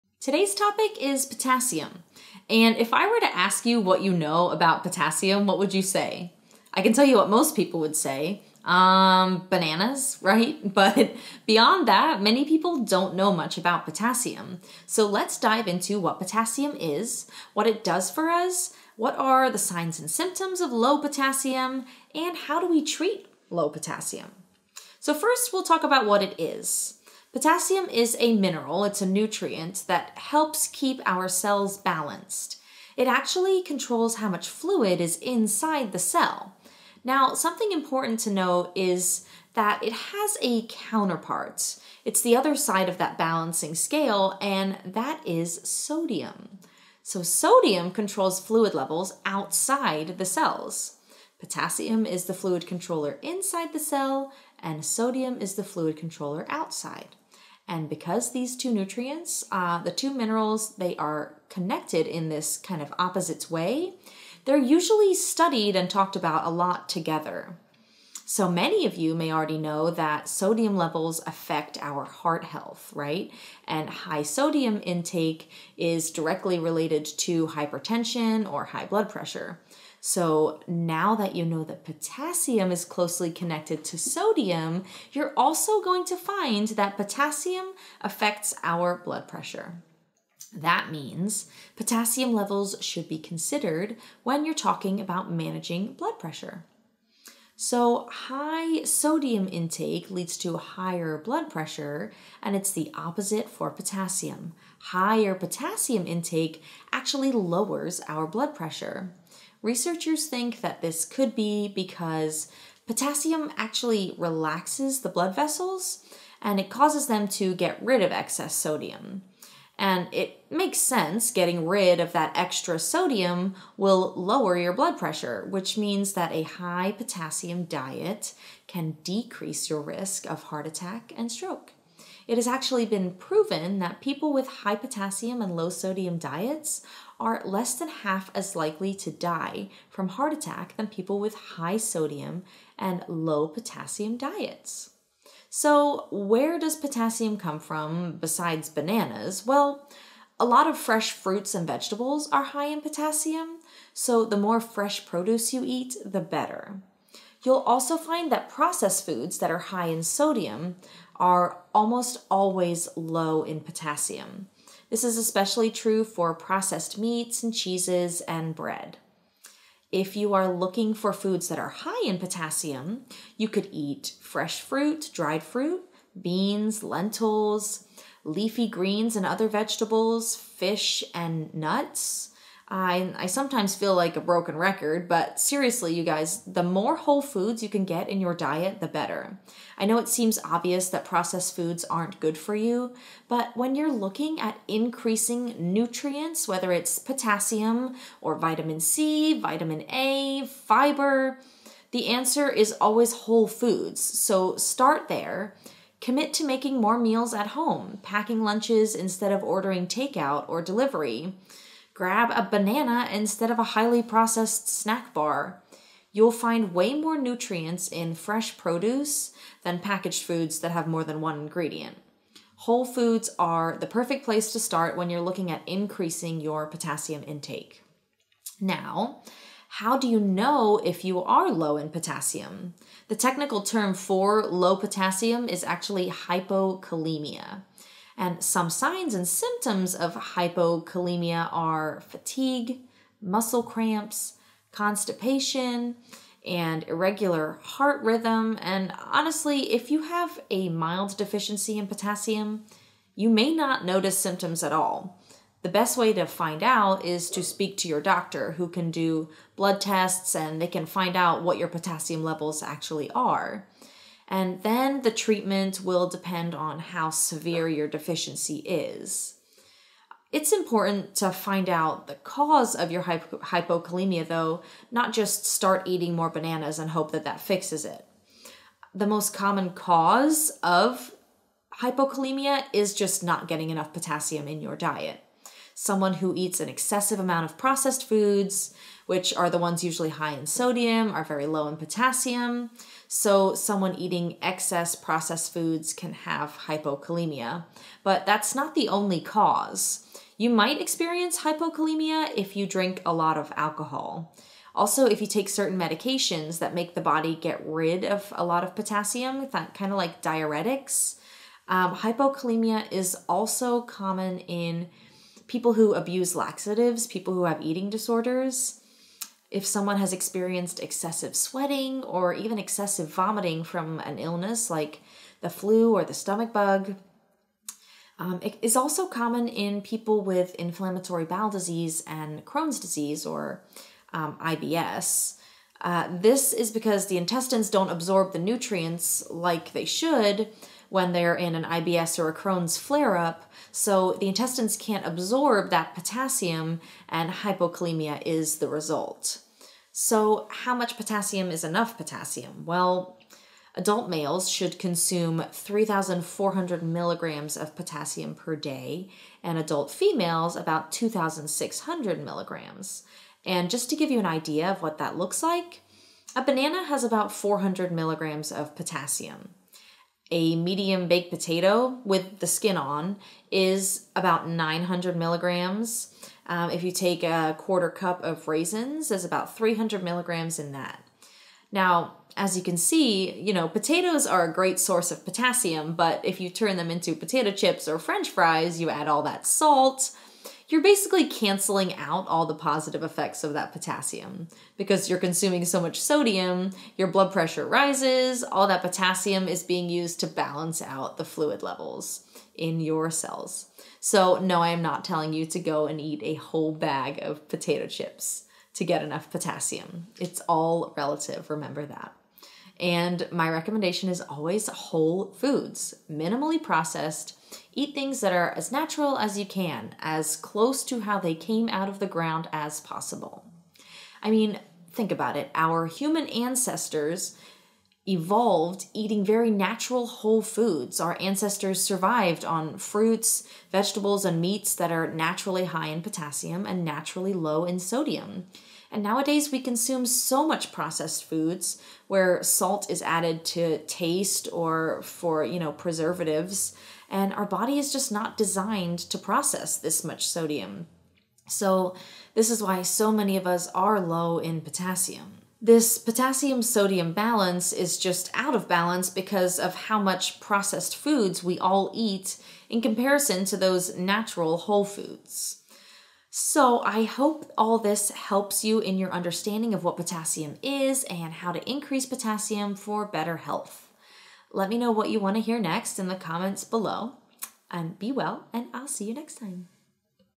Low Potassium: Signs, Symptoms, Causes, and Treatment
Potassium is a familiar word to most but not well understood by many. Find out what it is and why we need it in this video!

SUMMARY:
Potassium is a mineral that controls how much fluid is inside the cell. Potassium is the fluid controller inside the cells, and sodium is the fluid controller outside. Because these two nutrients are connected in this opposite way, they are usually studied and talked about together.

Potassium effects our body by lowering blood pressure. This means that a high potassium diets can decrease your risk of heart attack and stroke. Fresh and dried fruits, beans and lentils, leafy greens, fish, nuts and vegetables are high in potassium.

Some signs and symptoms of low potassium are fatigue, muscle cramps, constipation and irregular heart rhythm. Truthfully if you have a mild deficiency in potassium, you may not notice any symptoms at all. The best way to find out is to speak to your doctor who can do blood tests to find out your potassium levels.

The most common cause is just not getting enough potassium in your diet. You might also experience low potassium if you drink a lot of alcohol, take certain medications like diuretics and laxatives, excessive sweating or vomiting from an illness like the flu or a stomach bug, and inflammatory bowel diseases like Crohn's disease or IBS. The treatment will then depend on how severe your deficiency is.

CHAPTERS:
0:26 What is Potassium?
0:49 Potassium and sodium connection
1:26 Potassium and blood pressure
2:27 Sources of potassium
3:49 Low potassium signs and symptoms
4:40 Causes of hypokalemia (low potassium)
5:49 How much potassium do we need?
7:48 Potassium sodium balance